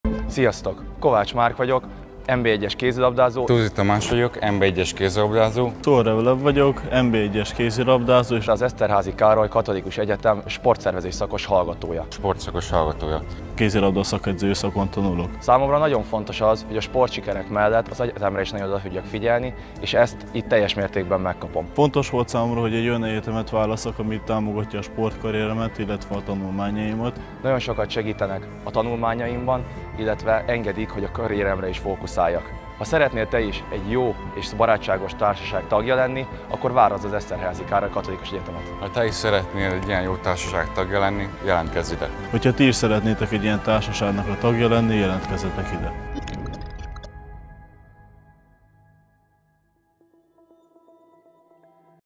Eszterházy Károly Katolikus Egyetem Sporttudományi Intézet - SBS Eger
🤾🏽‍♂️ Kovács Márk, Tóvizi Tamás és Szuharev Lev, az SBS-Eger férfi kézilabda csapat NBI-es kézilabdázói is az EKKE Sporttudományi képzéseit választották 🤾🏽‍♂️
👉🏼 a hallgatókra egyenként is figyelünk, támogatjuk tanulmányaikat, az élsportolók kiemelt figyelmet kapnak a tanulmányaik során.
Válaszd Te is az Eszterházyt!